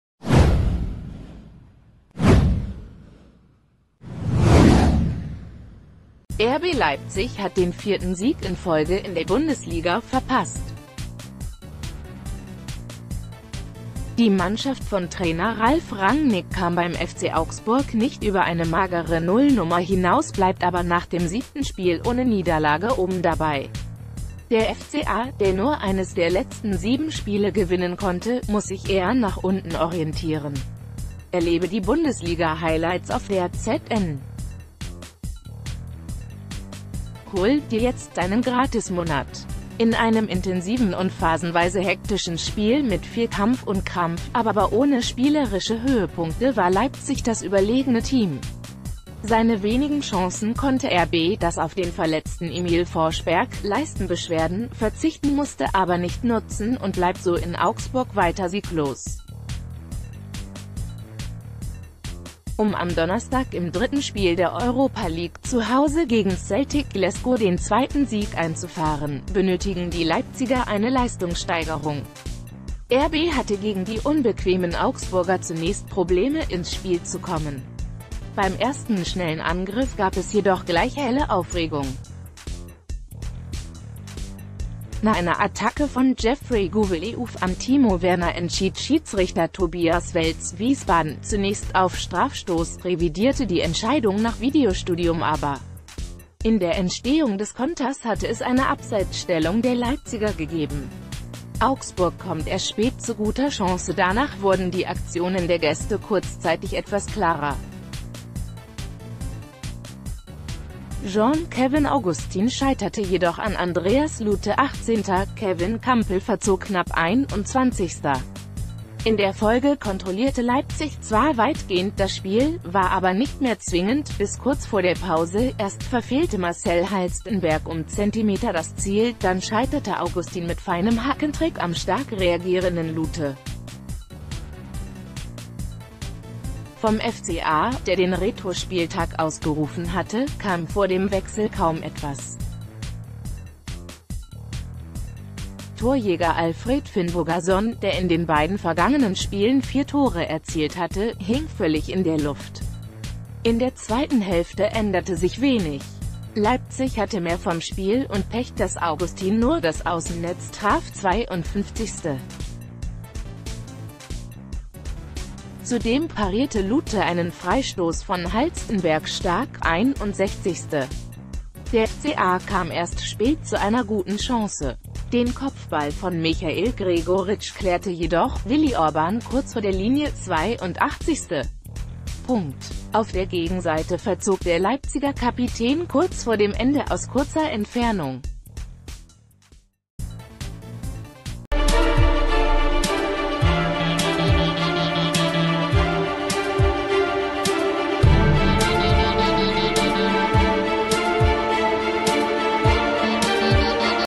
FC Augsburg vs. RB Leipzig Spielbericht, 20.10.18, Bundesliga
RB Leipzig hat den vierten Sieg in Folge in der Bundesliga verpasst. Die Mannschaft von Trainer Ralf Rangnick kam beim FC Augsburg nicht über eine magere Nullnummer hinaus, bleibt aber nach dem siebten Spiel ohne Niederlage oben dabei. Der FCA, der nur eines der letzten sieben Spiele gewinnen konnte, muss sich eher nach unten orientieren. Erlebe die Bundesliga-Highlights auf DAZN. Hol' Dir jetzt Deinen Gratismonat! In einem intensiven und phasenweise hektischen Spiel mit viel Kampf und Krampf, a...